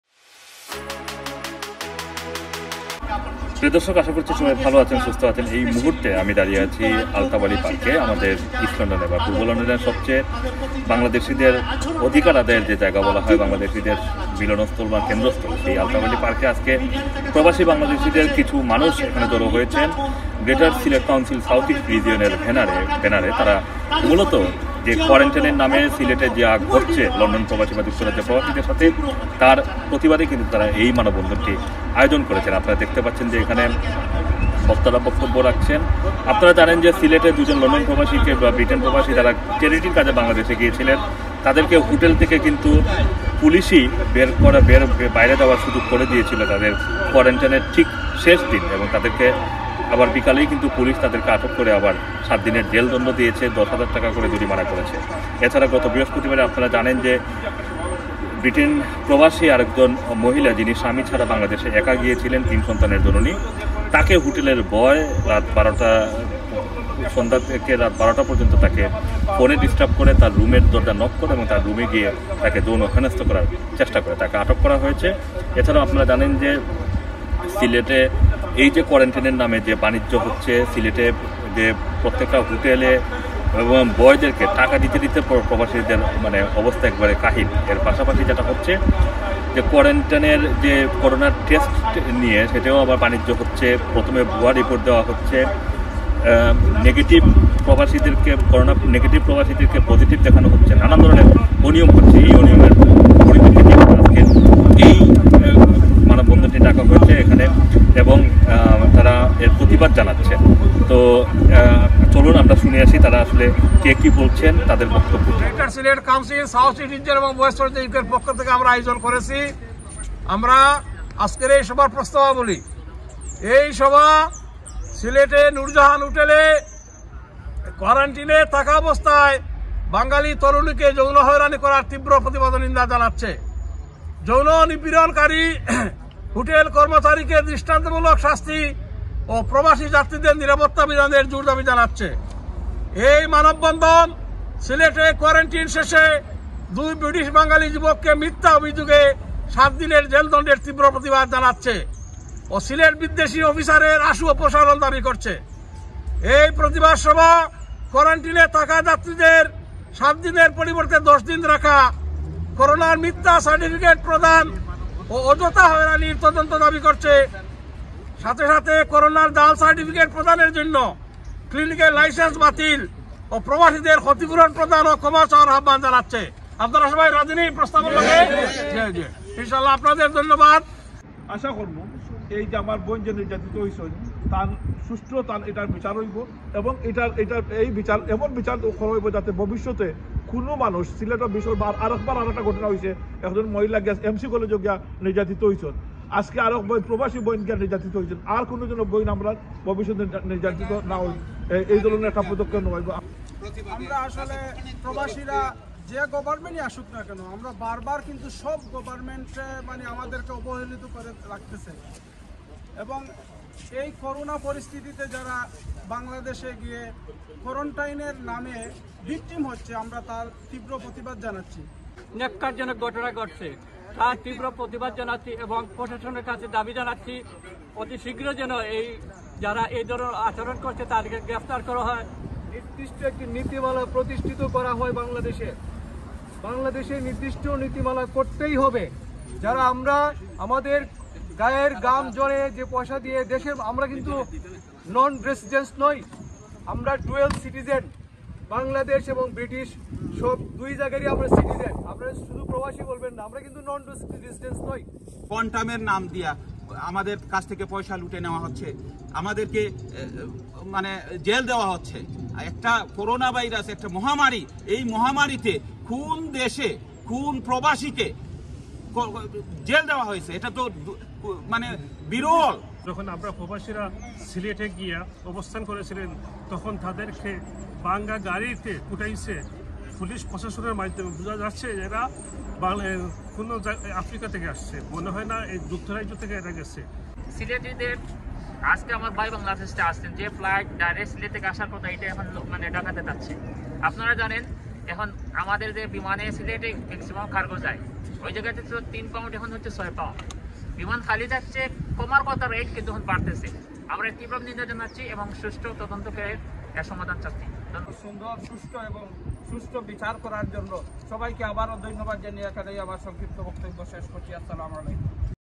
সিলেটে কোয়ারান্টিনের নামে হ. য়রানী ও বানিজ্য বন্ধে লন্ডনে মানববন্ধন | A S M Masum | Runner TV
সিলেটে কোয়ারান্টিনের নামে হয়রানী ও বানিজ্য বন্ধে লন্ডনে মানববন্ধন | A S M Masum | Runner TV